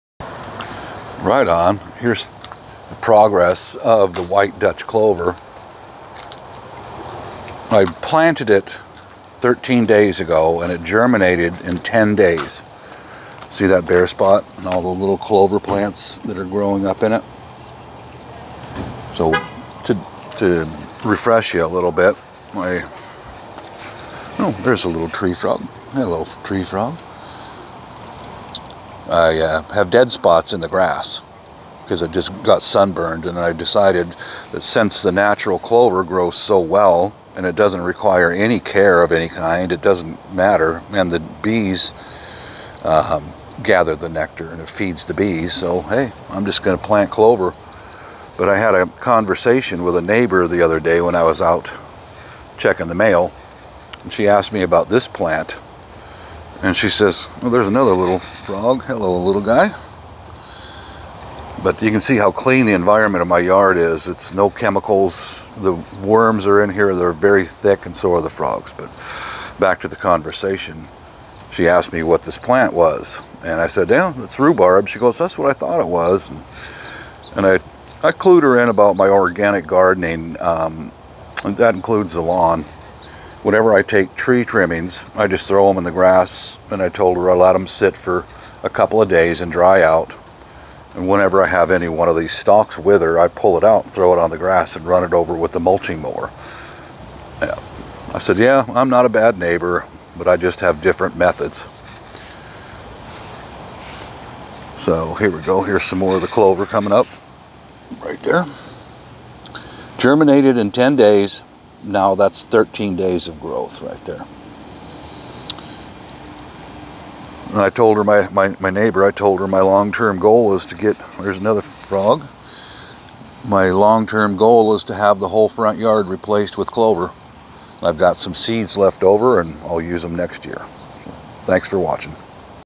white Dutch clover 13 days after planting
seeds germinated in 10 days,  this is 13 days of growth